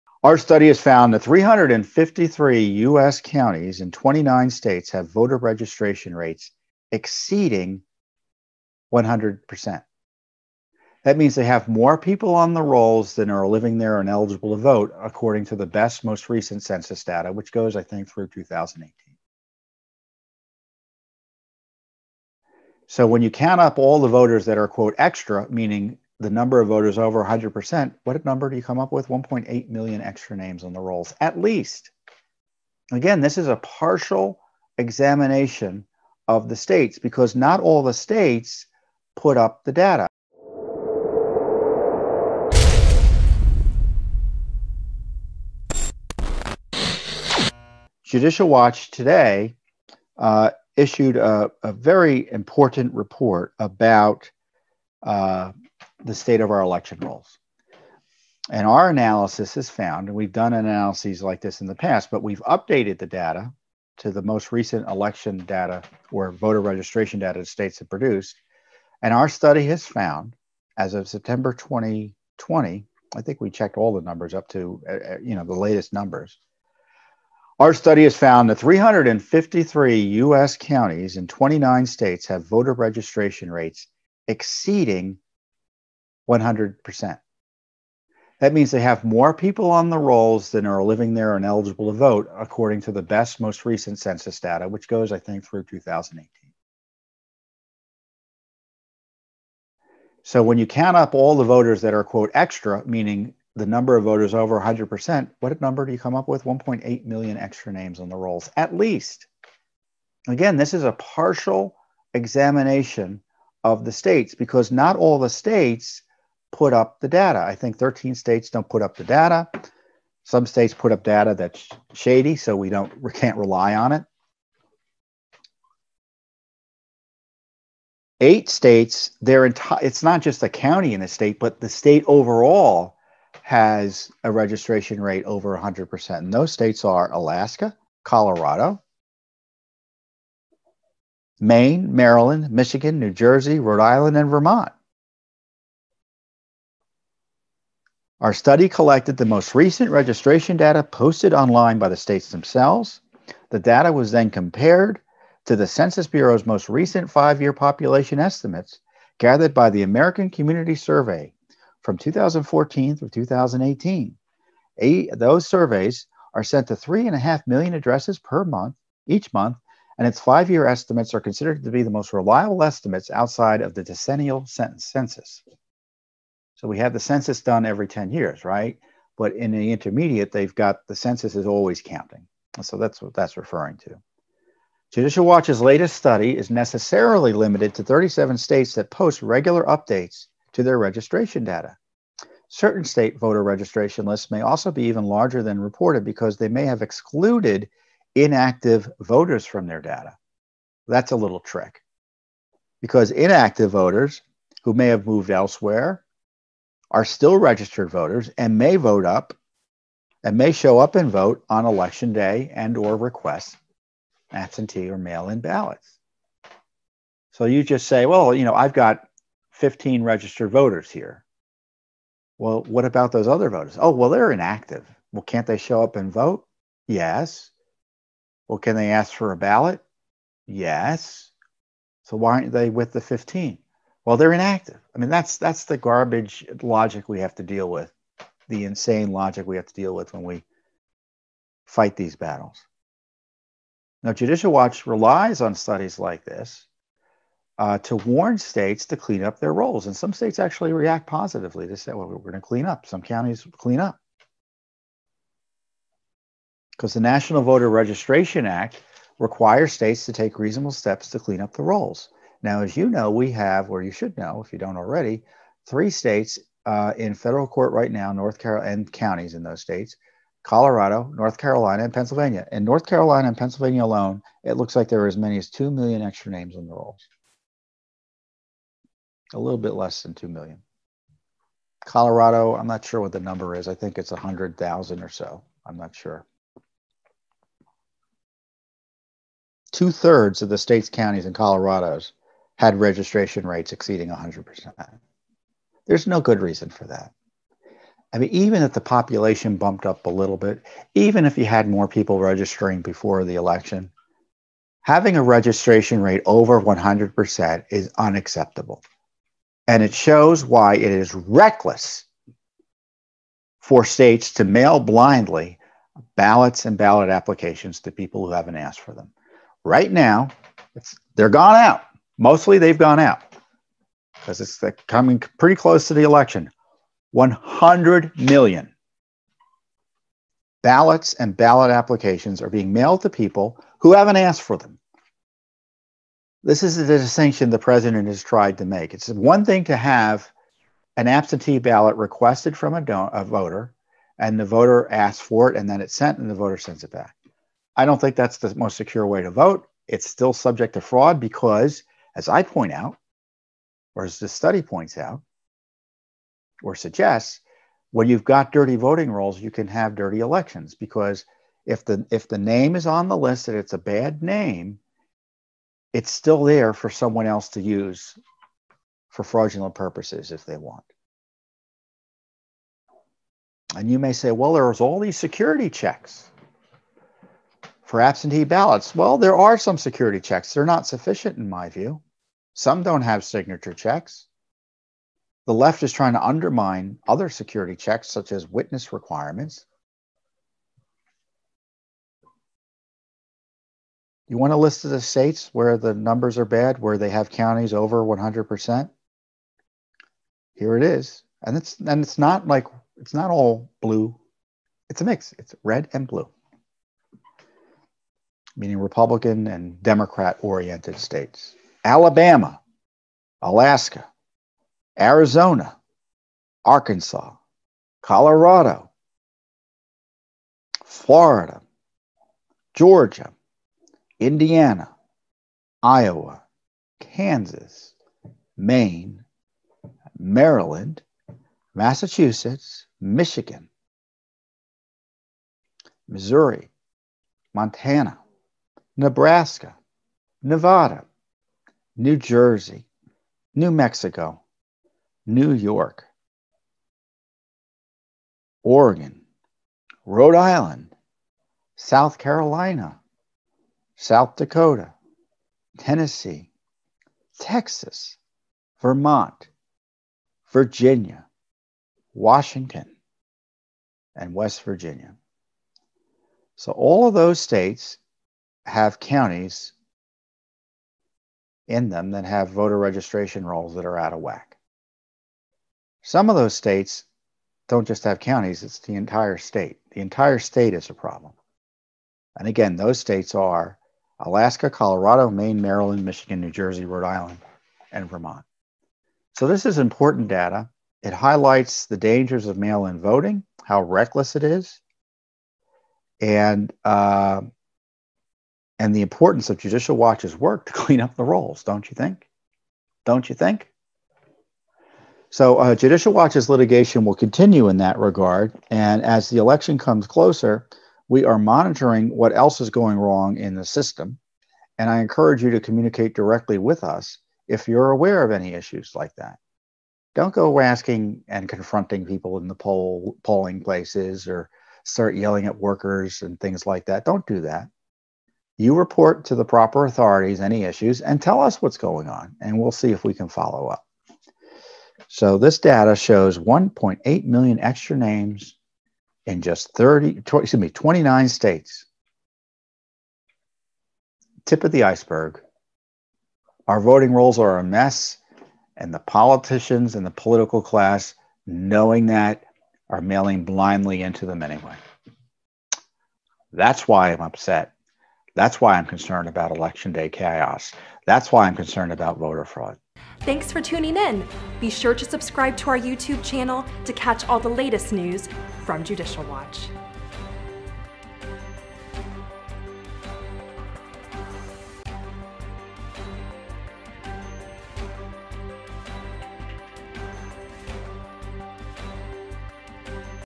New Study Finds 353 Counties in 29 States with Voter Registration Rates Exceeding 100%
Judicial Watch announced today that a September 2020 study revealed that 353 U.S. counties had 1.8 million more registered voters than eligible voting-age citizens. In other words, the registration rates of those counties exceeded 100% of eligible voters. The study found eight states showing state-wide registration rates exceeding 100%: Alaska, Colorado, Maine, Maryland, Michigan, New Jersey, Rhode Island, and Vermont.